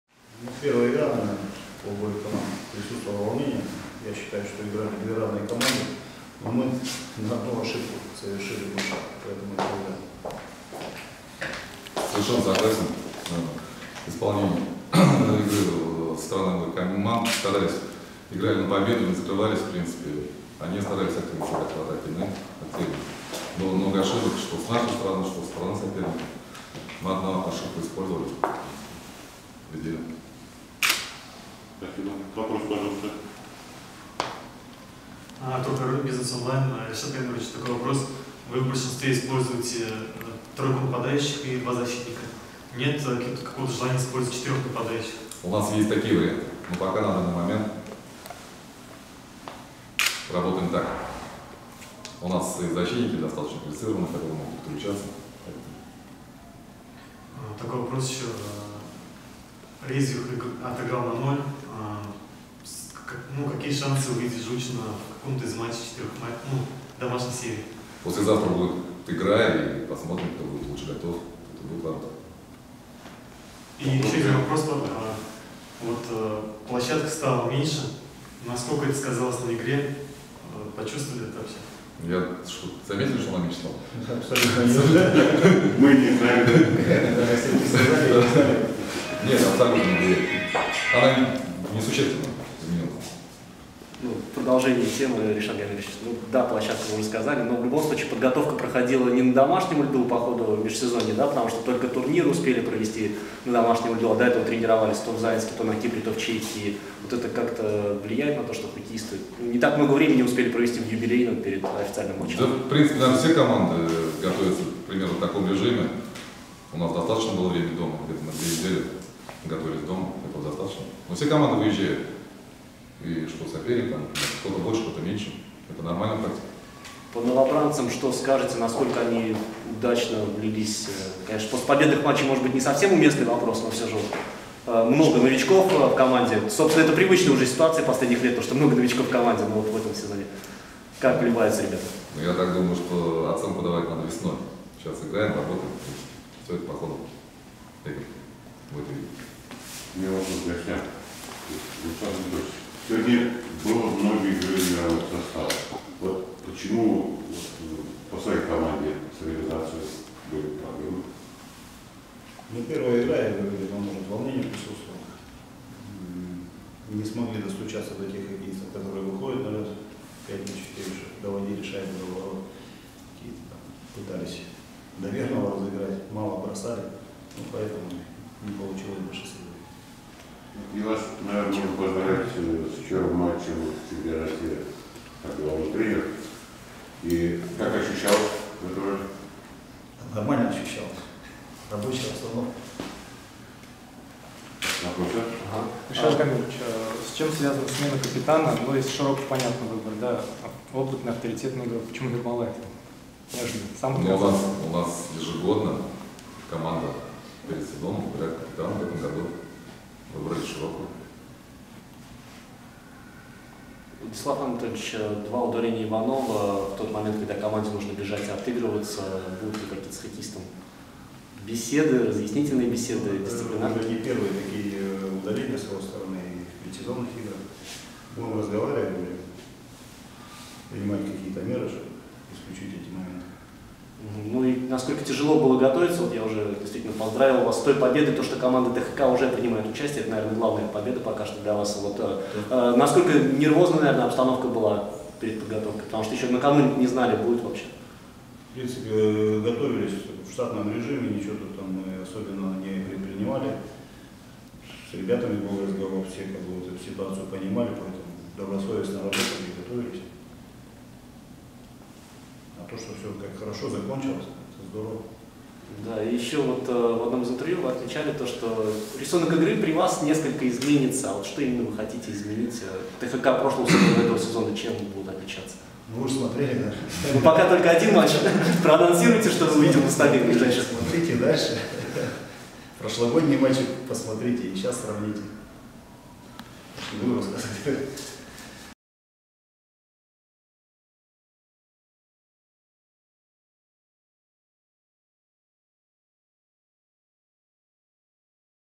Гимаев и Хромых после матча "Нефтяник" - "ТХК Тверь" (1:0)
Альметьевцы толпами идут на хоккей, спорят о новой и старой форме любимой команды, радуются возвращению Динара Хамидуллина и не замечают уменьшения размеров главной ледовой площадки... Спортивная редакция «БИЗНЕС Online» рассказывает о стартовом матче «Нефтяника» в новом сезоне ВХЛ.